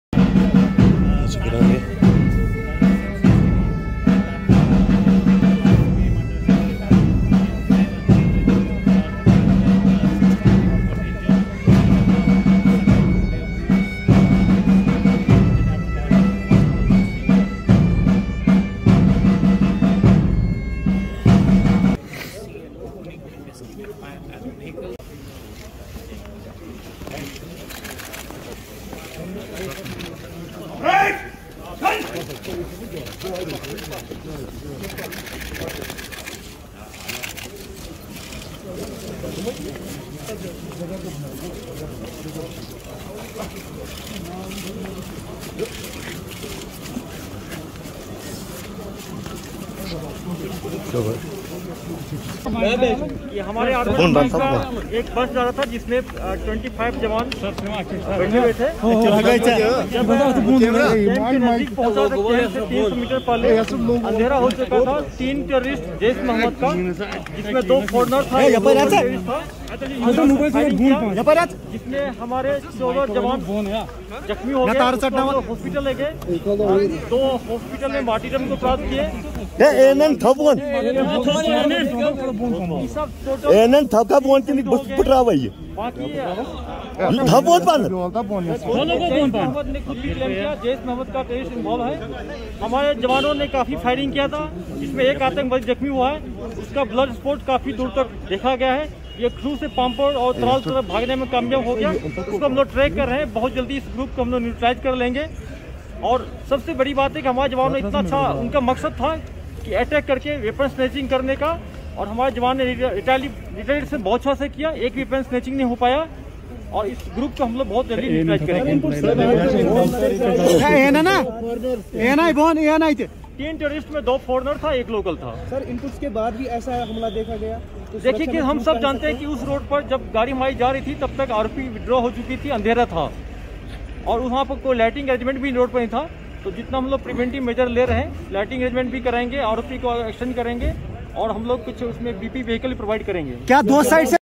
SRINAGAR: IGP Kashmir Vijay Kumar pays tribute to constable Rameez Ahmad Baba.
SRINAGAR: IGP Kashmir Vijay Kumar along with other police personnel pays tribute to constable Rameez Ahmad Baba who lost his life in yesterday's terrorist attack.